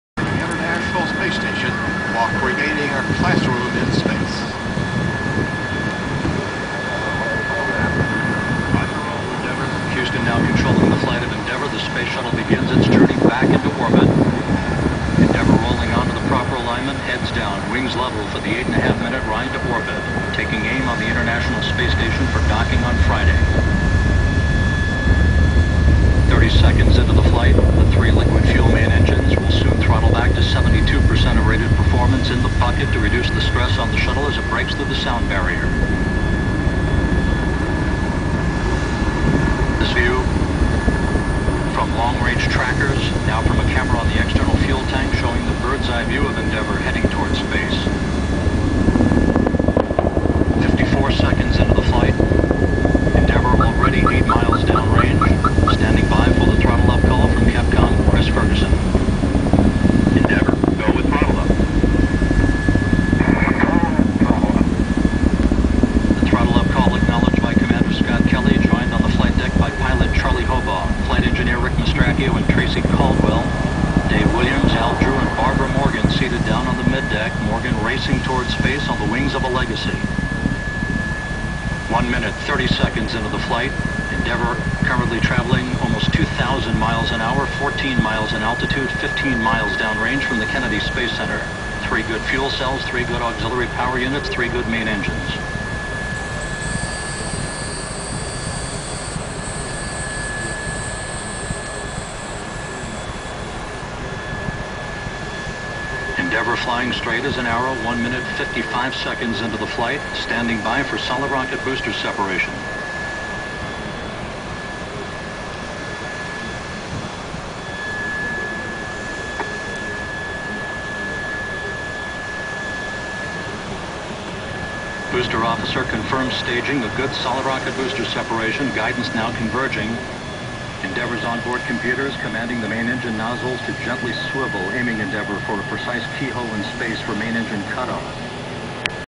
Space Shuttle Endeavor Launch 8.8.07
Video shot with Sony DSC-T100 4 miles from launch pad